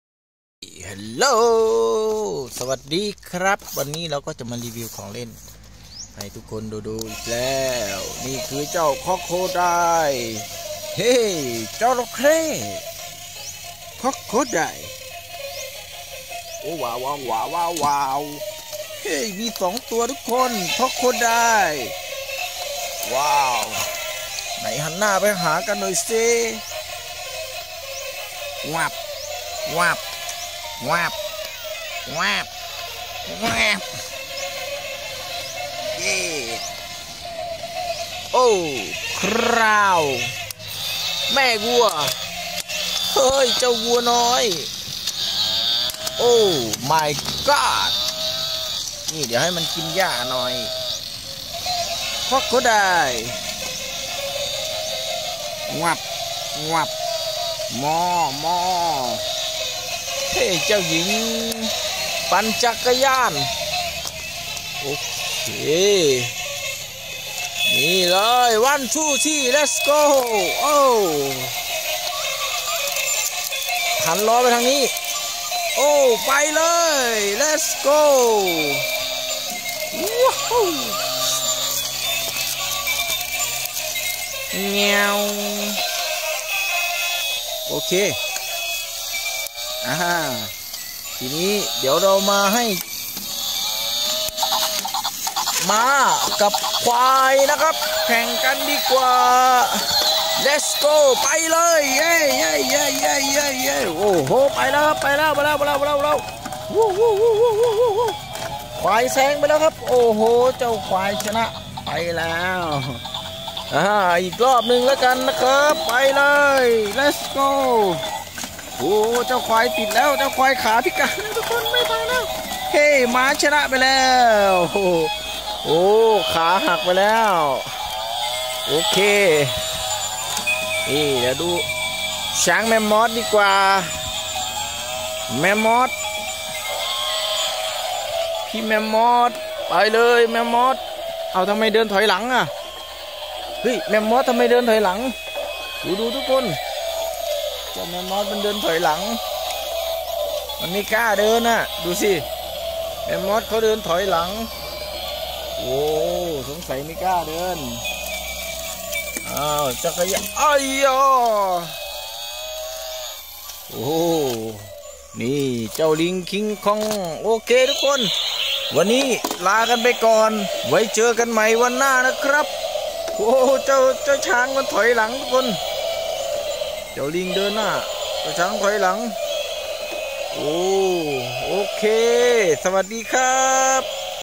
โชว์ของเล่น วัว มังกร เจ้าหญิงปั่นจักรยาน แมมมอธ เจ้าเป็ด ไดโนเสาร์ ช้างตัวใหญ่ ม้าลาย ยีราฟ
Show toys: cows, dragons, princesses on bicycles, mammoths, ducks, dinosaurs, large elephants, zebras, giraffes.